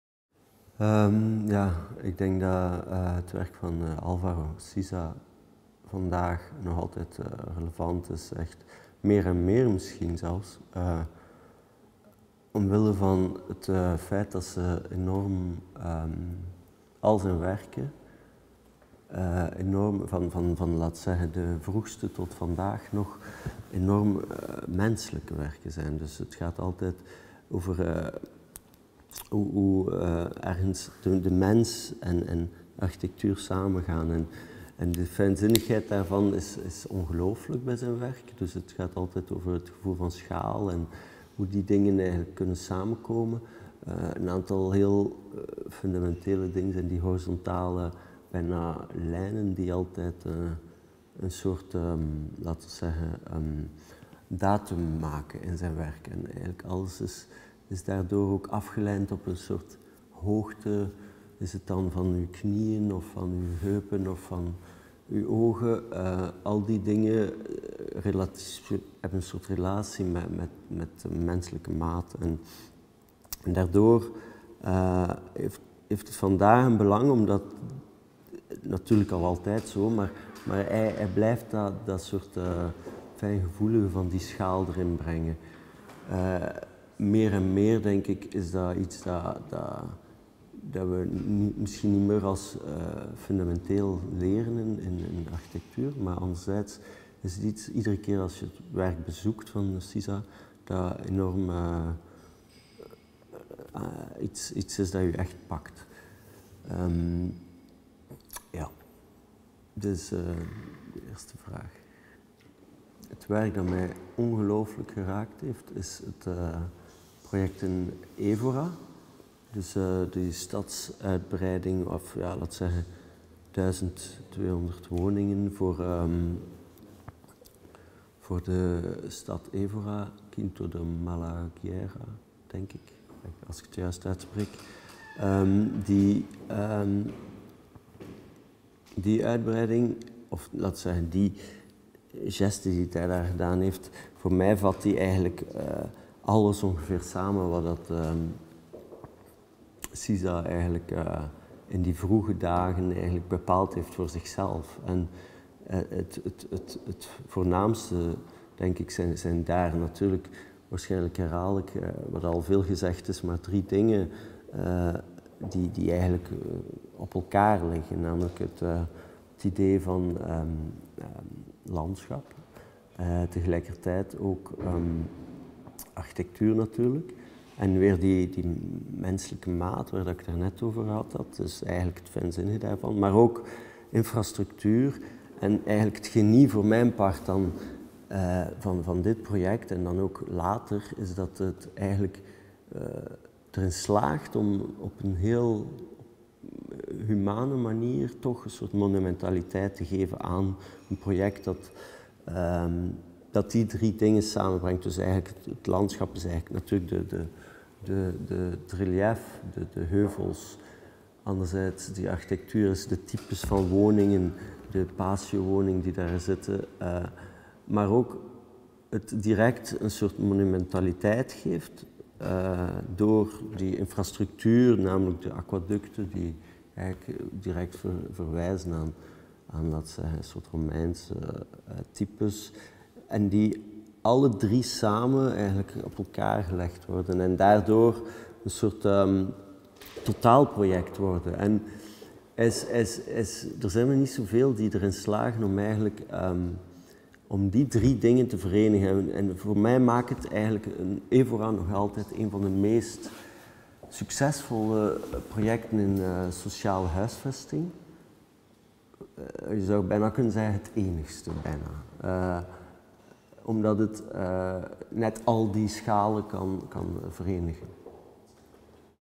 TESTEMUNHO DAVID VAN SEVEREN (BRUXELAS) - EXPOSIÇÃO SIZA (2019) | SERRALVES
Testemunho David Van Severen  (Bruxelas) - Exposição Siza (2019)

Nome: Álvaro Siza
Disciplina: tão pouca quanto possível

Esta nota confessional - certo dia escrita por Álvaro Siza na guarda interior de um dos seus cadernos de desenho, de formato escolar - serviu de ponto de partida para esta exposição comemorativa do 20.º aniversário do Museu de Arte Contemporânea de Serralves.
Álvaro Siza:in/disciplina revela-nos a salutar inquietude e a insubmissão do seu método criativo que, forjado no cruzamento entre saberes, culturas, geografias, obras e autores, sustentou, ao longo de mais de seis décadas, um constante questionamento da arquitetura a partir, simultaneamente, do que está dentro e fora da disciplina.
Com base em trinta projetos realizados entre 1954 e 2019 (construídos ou não), a exposição percorre a trajetória de Álvaro Siza, desde o período da sua formação até à sua plena afirmação autoral, através das suas leituras, dos seus cadernos de esquissos e registos de viagem, dos retratos que dela fizeram fotógrafos e amigos, das publicações seminais que as publicaram e do testemunho pessoal de muitas personalidades que com ela se cruzaram ao longo do tempo.

Curadores da exposição: Nuno Grande e Carles Muro

A exposição é organizada pela Fundação de Serralves - Museu de Arte Contemporânea, Porto, com a contribuição fulcral do seu Arquivo Álvaro Siza, do Álvaro Siza Fonds do Canadian Centre for Architecture (CCA), Montréal, e do Arquivo Álvaro Siza da Fundação Calouste Gulbenkian (FCG), Lisboa.